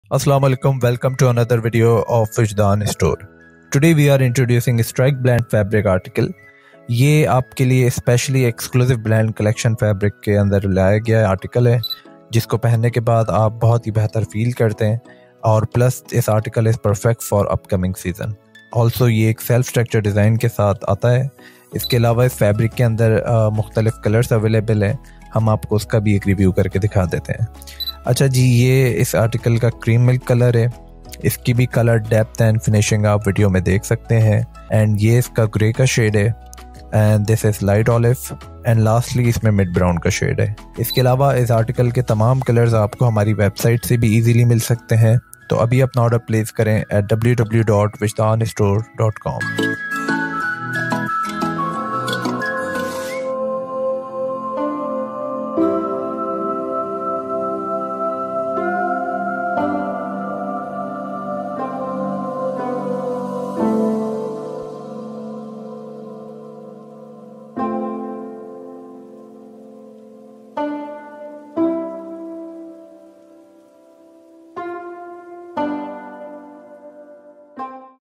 Wijdan Stores Presenting Woollen Collection Strike Blended
Introducing our Strike Wash & Wear Men's Unstitched Fabric - the epitome of winter elegance and comfort. Crafted with precision using high-quality microfibers, this fabric is your perfect companion for the chilly season. Each box packaging contains a piece of luxury waiting to be tailored into a masterpiece. Embrace warmth, style, and convenience with Strike Wash & Wear, designed to make your winter wardrobe truly exceptional.